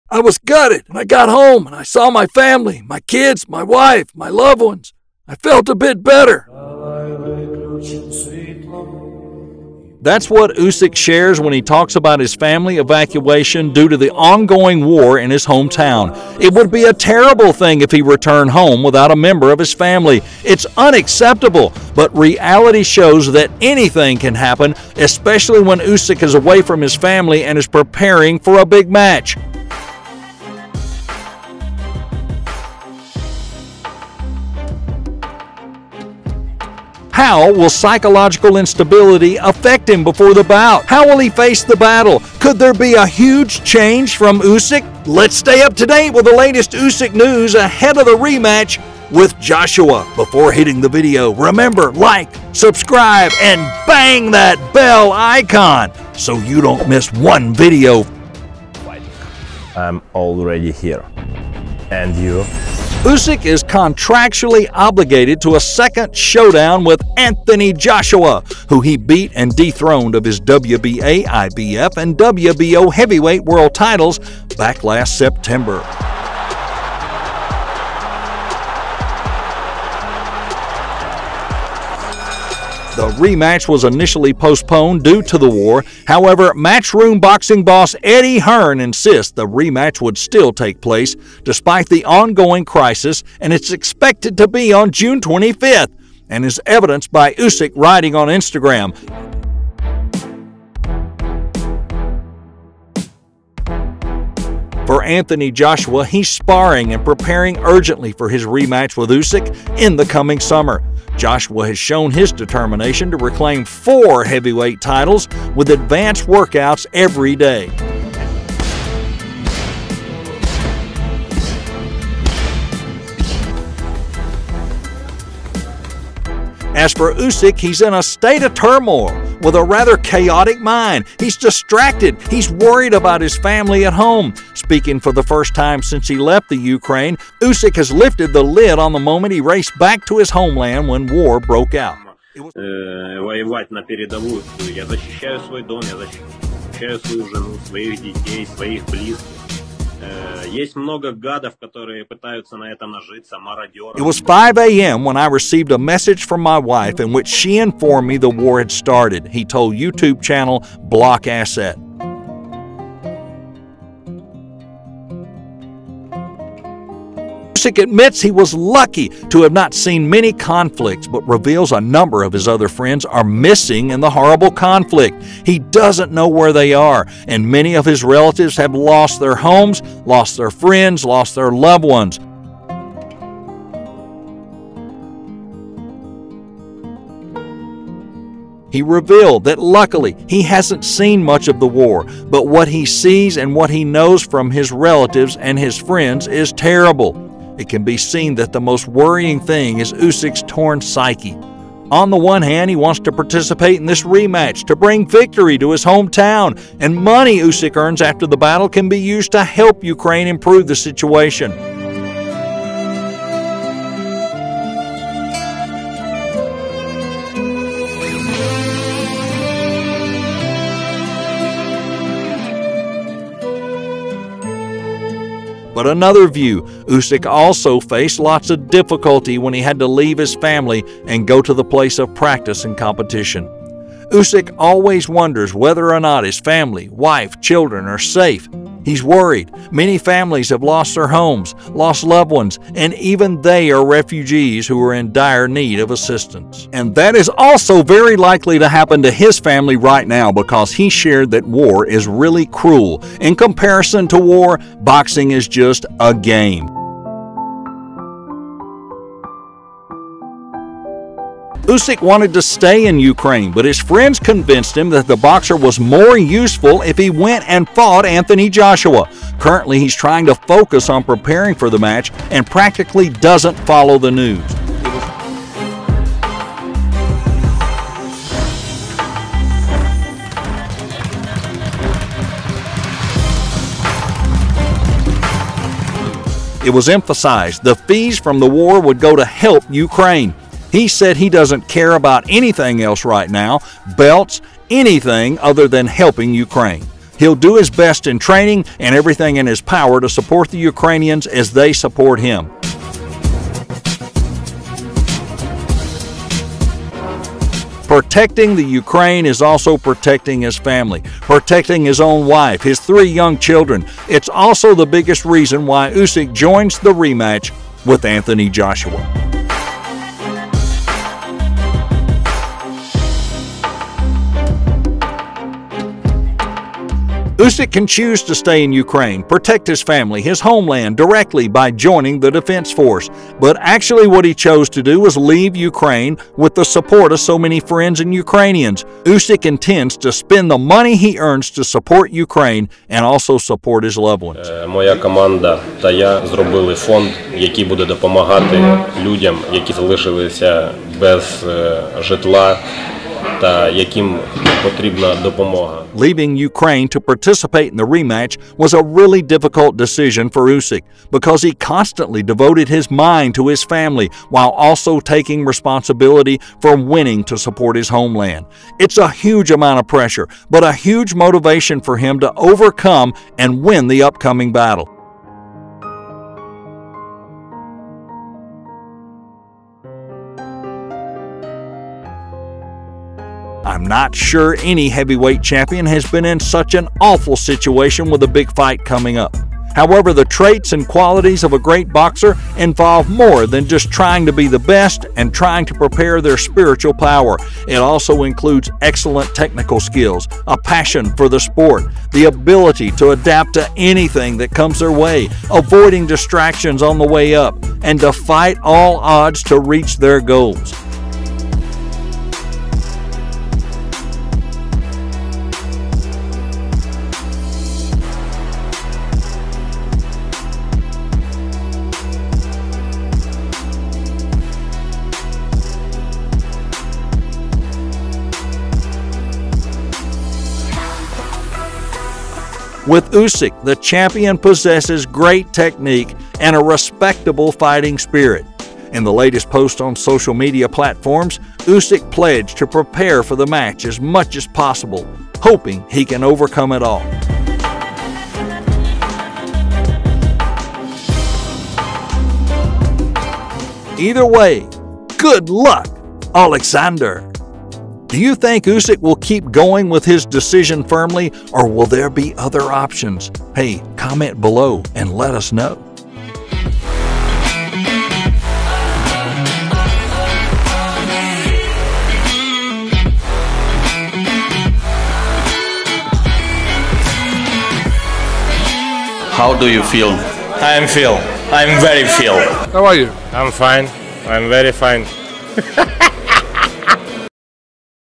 Oleksandr USYK Is Facing A Major Crisis. Will He Continue To Fight Anthony Joshua? Boxing Tonight
Latest updates about the rematch Uysk Joshua 2022
Is anxiety for family a driving force or pressure for Usyk before the rematch?

“'I was gutted”
“But when I got home, and I saw my family, my kids, my wife, my loved ones, I felt a bit better.”(reader pays attention to change the tone of voice to sound like Oleksandr Usyk)
That's what Uysk shares when he talks about his family's evacuation due to the ongoing war in his hometown. It would be a terrible thing if he returned home without a member of his family. That is unacceptable. But reality shows that anything can happen, especially when Usyk is away from his family and is preparing for a big match.
How will psychological instability affect him before the bout?
How will he face this battle? Could there be a huge change from Usyk?
Let's stay up to date with the latest Usyk news ahead of the rematch with Joshua.
 Usyk is contractually obligated to a second showdown with AJ, who he beat and dethroned of his WBA, IBF, and WBO heavyweight world titles back last September. The rematch was initially postponed due to war however Matchroom Boxing boss Eddie Hearn insists that the rematch would still take place despite the ongoing crisis and is expected to be on 25th June. And as evidenced by Usyk writing on Instagram: “I decided to start preparing for a rematch with Anthony Joshua. A large number of my friends support me, all the rest of the good and peace, Thank God for everything."
For Anthony Joshua, he's sparring and preparing very urgently for his rematch with Usyk in the coming summer. Joshua has shown his determination to reclaim 4 heavyweight titles with advanced workouts every day.
As for Usyk, he is in a state of turmoil with a rather chaotic mind, distracted for be worried about his family at his local. Speaking for the first time since he left Ukraine, Usyk has lifted the lid on the moment he raced back to his homeland when war broke out.
'It was 5 am when I received a message from my wife in which she informed me that the war started,' he told YouTube channel Blockasset.
'I was gutted, I could not understand what was going on, but when I got home and saw my family, my kids and my wife, my loved ones, I felt a bit better.'
(reader pays attention to change the tone of voice to sound like Oleksandr Usyk)
Usyk admits he was lucky to have not seen many conflicts but reveals a number of his other friends are missing in the horrible conflict. He does not know where they are now. Many of his relatives lost their homes, lost their friends, and loved ones. He revealed that he luckily did not see much of war. But what he sees and what he knows from his relatives and his friends is terrible. It can be seen that the most worrying thing is Usyk's torn psyche. On the one hand, he wants to participate in the rematch so he can bring victory to his hometown, and the money Usyk earns after the battle can be used to help Ukraine improve the situation. In another view, Usyk also faced a lot of difficulties when he had to leave his family to go to the place of practice and competition. Usyk always wonders whether or not his family, wife, and children are safe. Many families have lost their homes, lost loved ones and even they are refugees who are in dire need of assistance. And that is also very likely to happen to his family right now because he shared that war is really cruel, in comparison to war, boxing is just a "game".